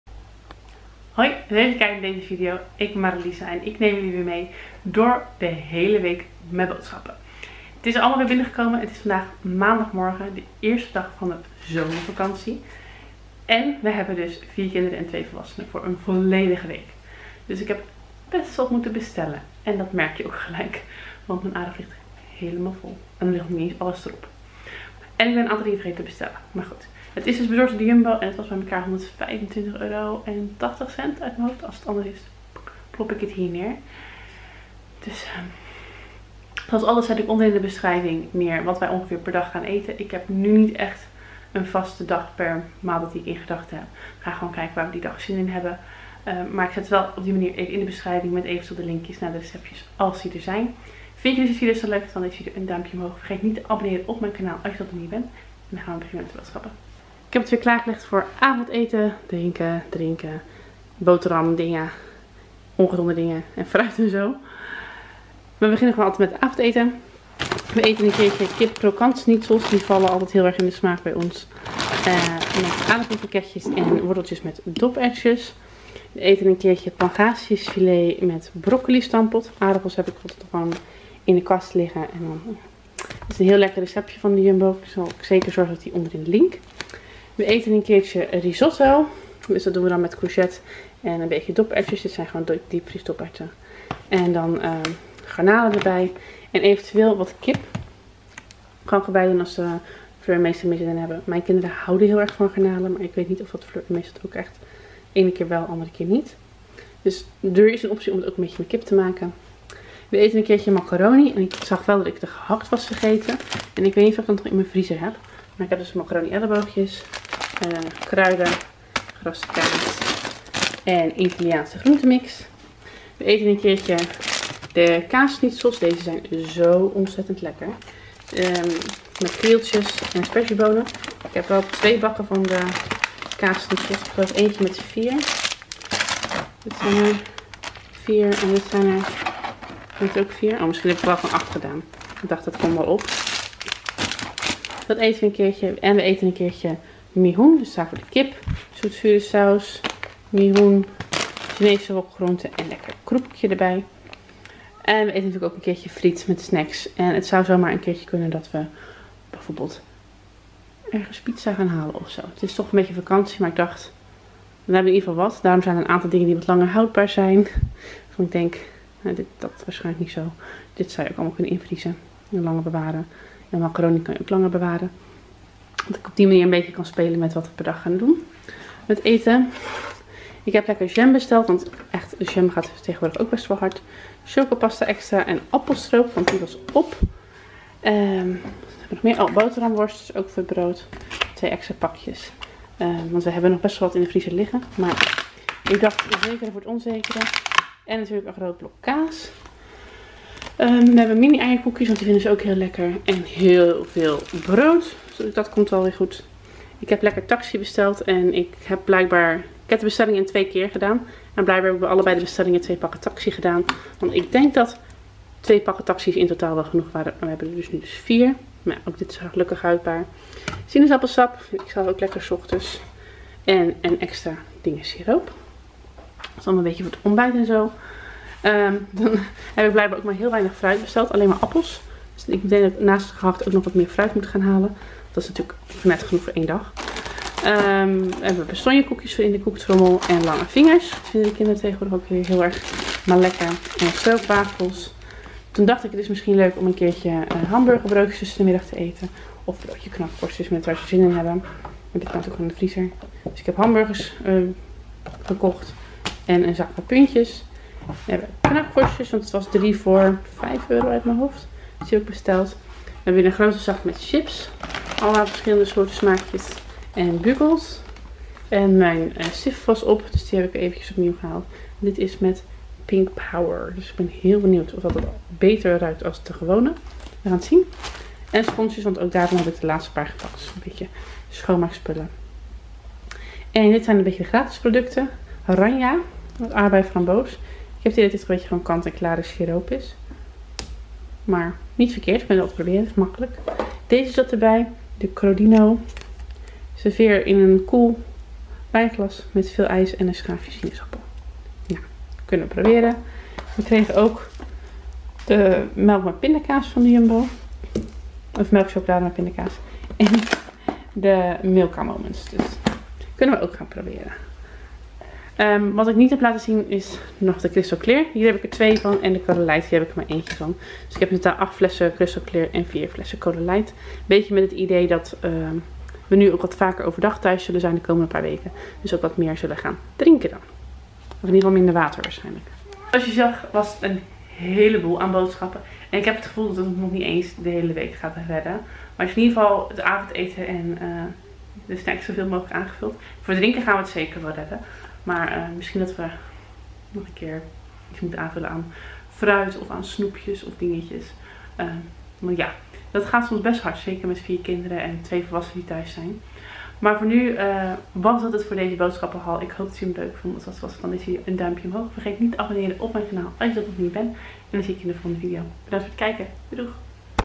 WAT ETEN WIJ IN DE ZOMERVAKANTIE • Boodschappen shoplog #23 | Mariliza Pauwels
Maandag: Kipkrokant schnitzel met doperwten, worteltjes en aadappelkroketten
Dinsdag: Pangasiusfilet met broccoli stampot
Woensdag: Risotto met garnalen
Donderdag: Macaroni
Vrijdag: Kaasschnitzel met krieltjes en sperziebonen
Zaterdag: Friet met snacks
Zondag: Mihoen met kip